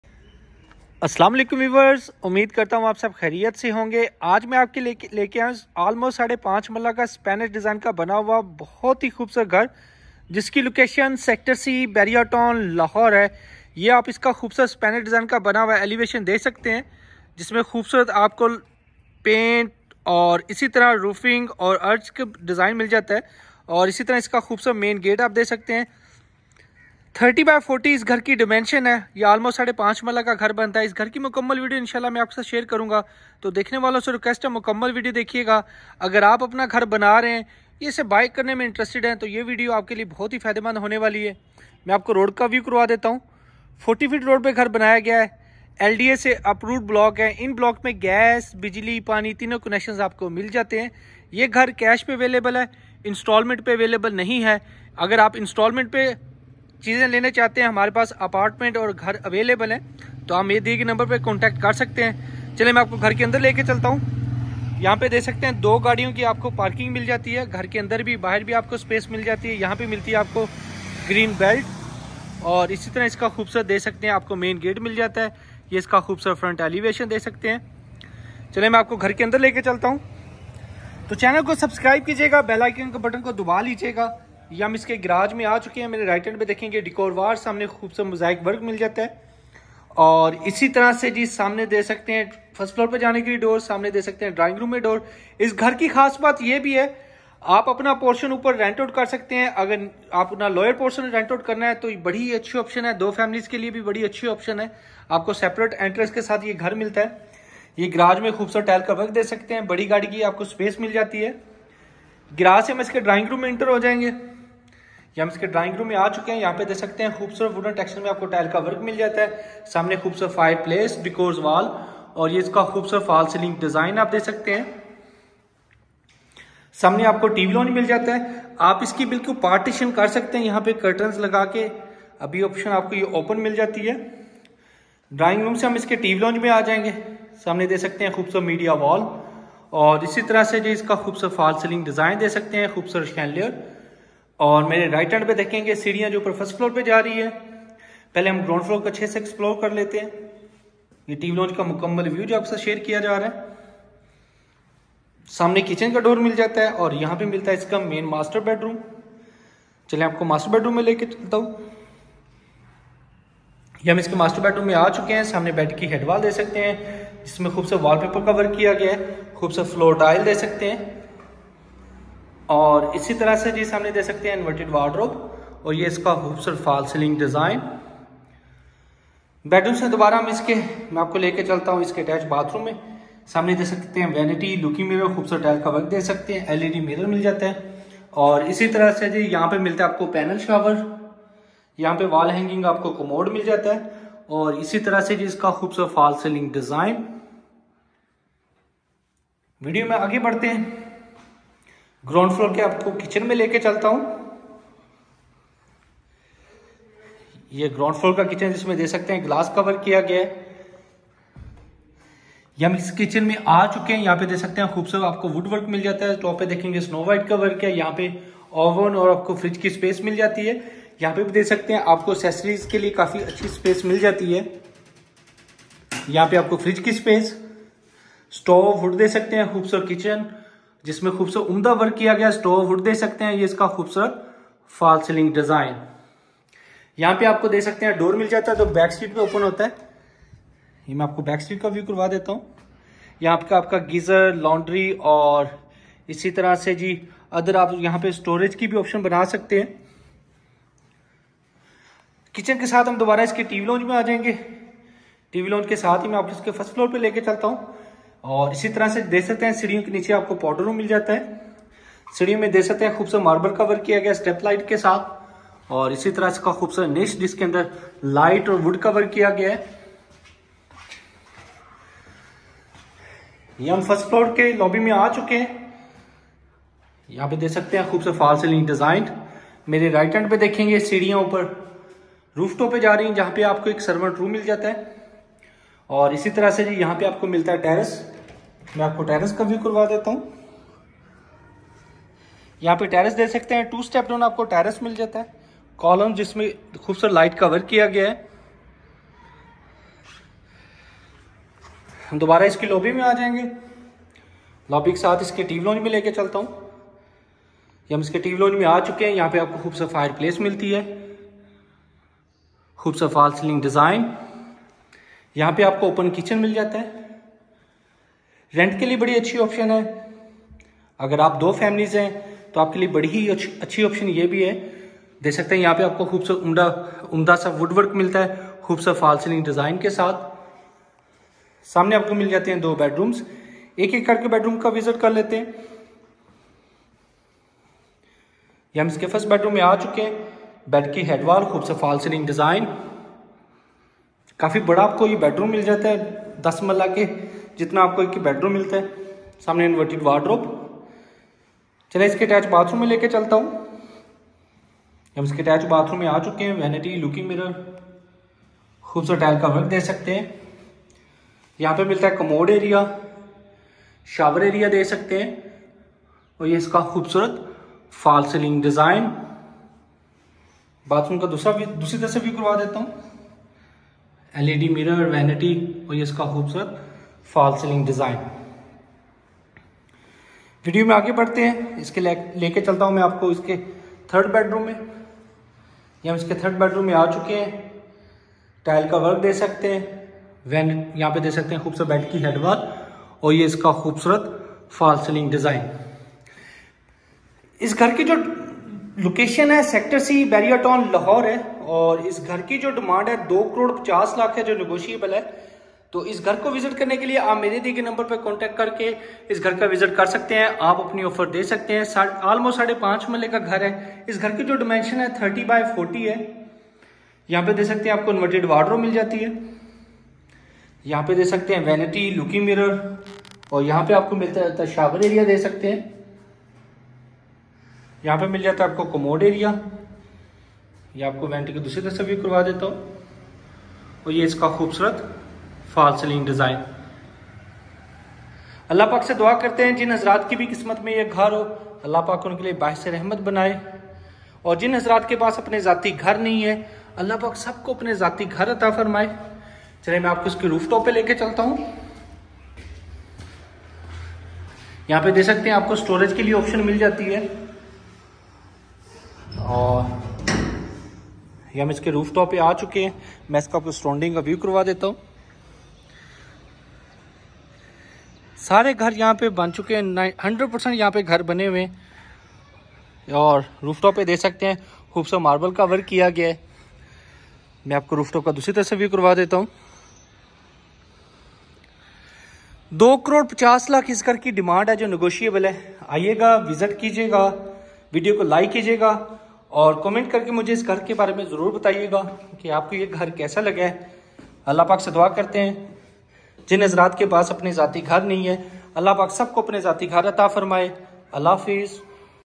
5.33 Marla Spanish Design House For Sale In Bahria Town Lahore For More Info 03012220989
5Marla Beautiful Spanish Design  House
For Sale Bahria Town Lahore

House Features:
-3 Bedrooms with attached Washrooms.
-2 TV Lounge.
-1 Drawing Room
-2 Kitchens
-1 Powder Room.